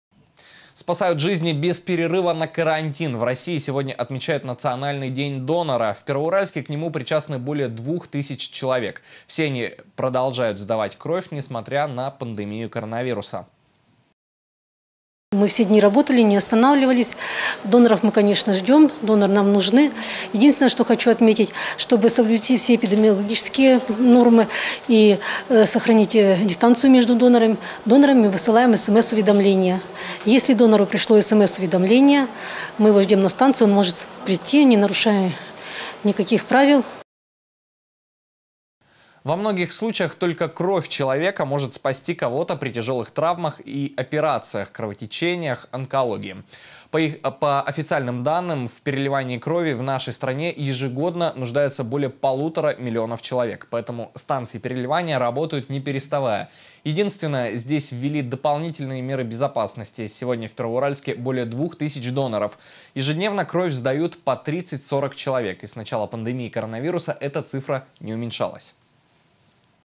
В России отмечают национальный день донора
Спасают жизни без перерыва на карантин. В России сегодня отмечают Национальный день донора. В Первоуральске к нему причастны более двух тысяч человек. Все они продолжают сдавать кровь, несмотря на пандемию коронавируса.

Ирина ПОНОМАРЕВА, заведующая отделом комплектования донорских кадров областной станции переливания крови /г. Первоуральск/: «Мы все дни работали, не останавливались. Доноров мы, конечно, ждем, доноры нам нужны. Единственное, что хочу отметить: чтобы соблюсти все эпидемиологические нормы и сохранить дистанцию между донорами - высылаем смс-уведомление. Если донору пришло уведомление - мы его ждем на станции, он может прийти, не нарушая никаких правил».

Во многих случаях только кровь человека может спасти кого-то при тяжелых травмах и операциях, кровотечениях, онкологии. По официальным данным в переливании крови в нашей стране ежегодно нуждаются более полутора миллионов человек. Поэтому станции переливания работают не переставая. Единственное, здесь ввели дополнительные меры безопасности. Сегодня в Первоуральске более двух тысяч доноров. Ежедневно кровь сдают по 30-40 человек. И с начала пандемии коронавируса эта цифра не уменьшилась.